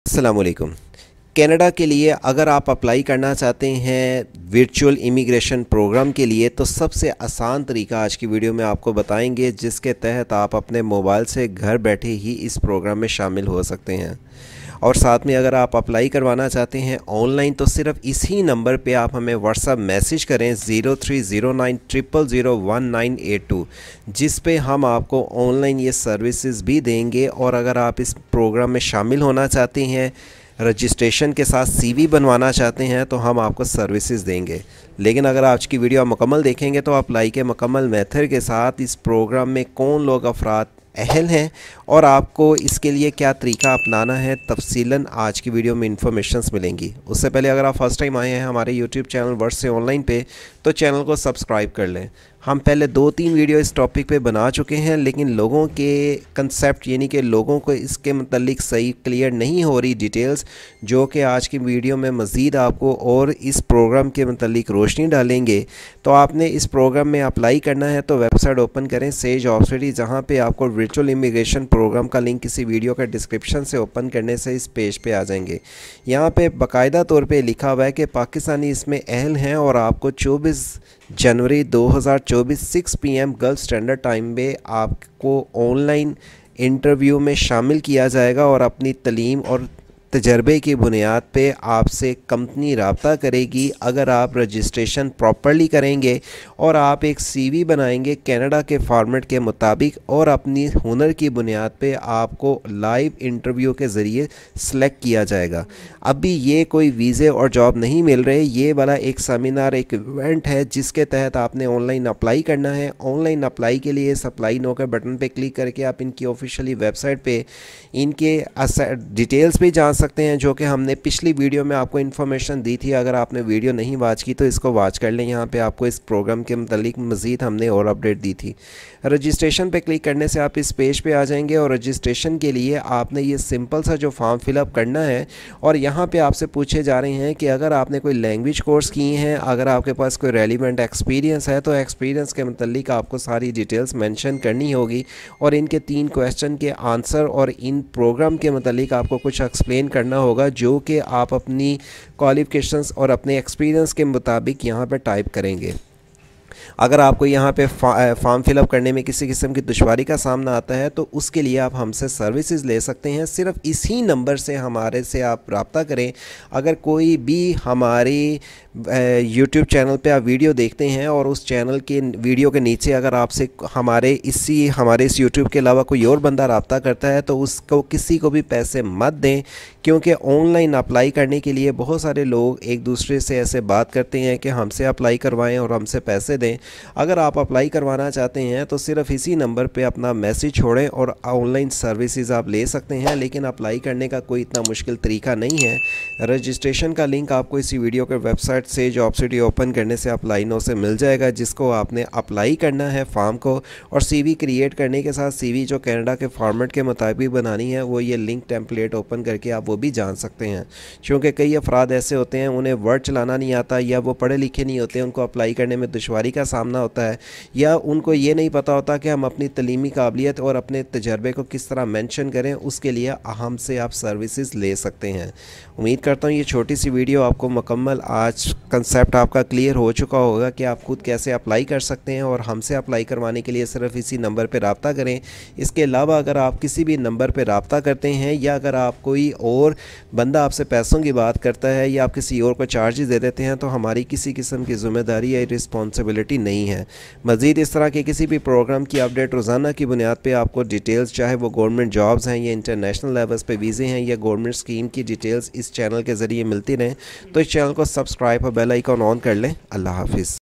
How to Apply Canada Virtual Immigration Program - Virtual Immigration Fair 2024 Registration Online
Virtual Immigration Fair 2024 – In orchestrating a digital symposium slated for January 24, 2024, the governmental body of Newfoundland and Labrador is orchestrating an avant-garde initiative. The overarching objective is to allure individuals from East Asia, Southeast Asia, and the Oceania region, fostering immigration and employment prospects within the Canadian landscape.